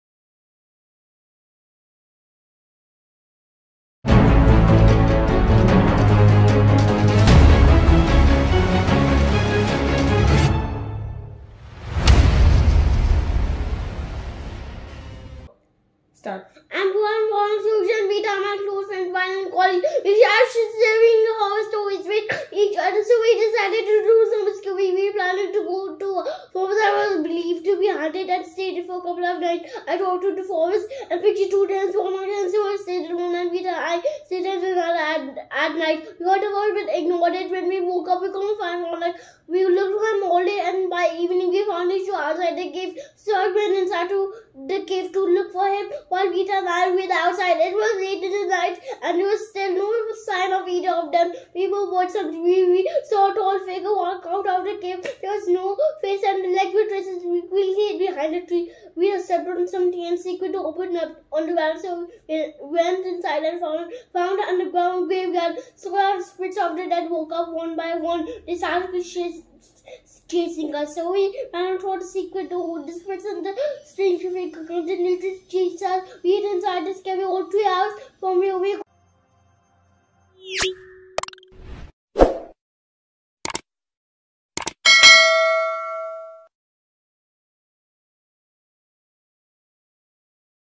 Maximum words read in a minute by a kid
The record for reading the maximum number of words in one minute was set by Aryan Uppal (born on March 4, 2017) of Panipat, Haryana. He read 227 words in one minute at the age of 4 years, 10 months and 14 days, as confirmed on January 18, 2022.